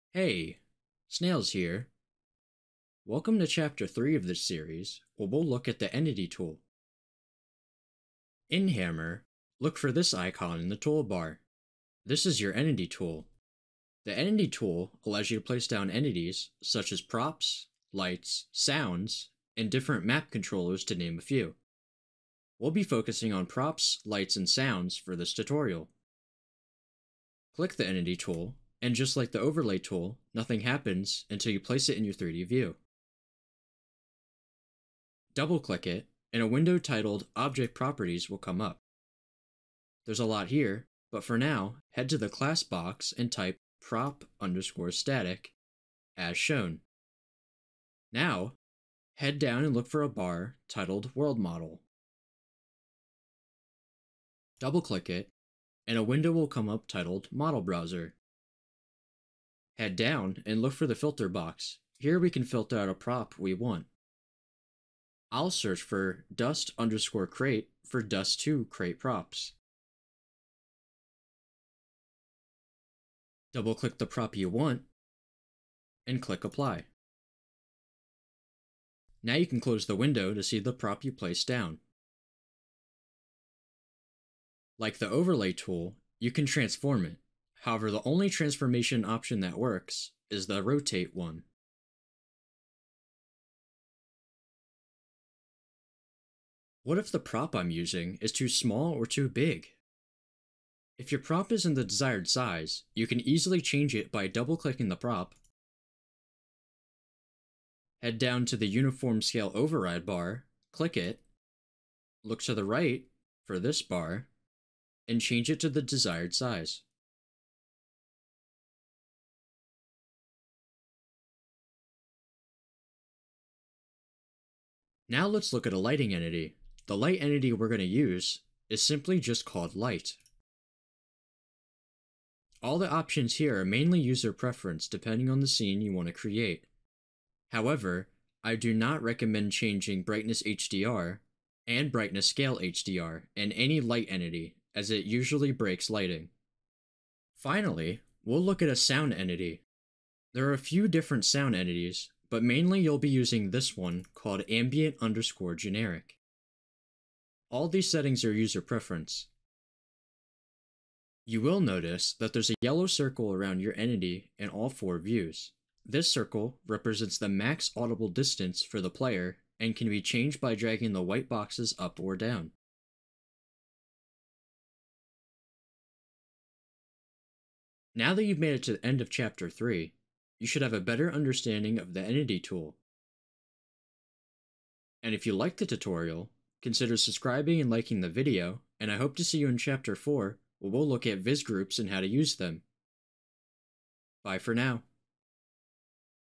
Chapter 3 | Hammer Tutorial Series
Chapters:
0:00 Intro
0:08 In Hammer
0:11 Entity Tool
0:41 Placing down props
1:35 If the prop is too big or too small
2:04 Light entity
2:27 Sound entity
2:57 Outro

Link to Hammer++ Download link
Link to Developer Valve Wiki for map creation

More info on the Tools covered.
[Ignore tags]
csgo,csgo mapping,csgo mapping tutorial,hammer++,hammer editor,mapping in hammer++, mapping in hammer